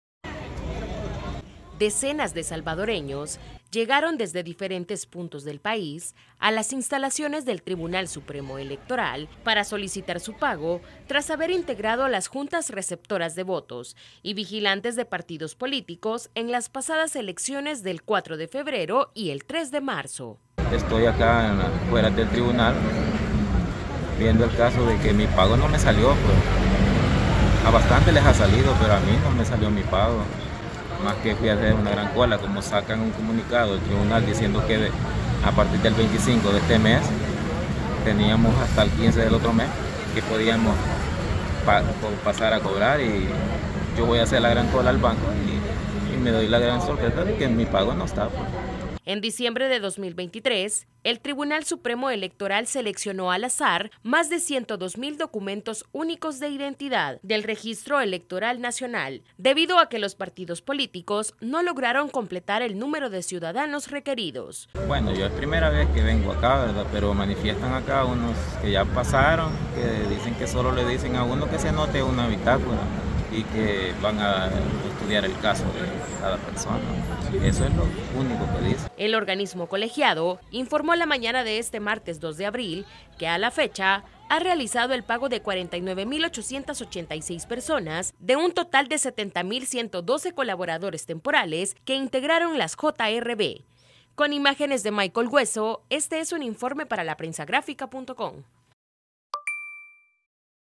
Salvadoreños exigen al TSE el pago por integrar Juntas Receptoras de Votos en elecciones 2024
El Tribunal Supremo Electoral informó la mañana de este martes 2 de abril que a la fecha ha realizado el pago a más de 49,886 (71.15%) personas de un total de 70,112 colaboradores temporales que integraron las JRV.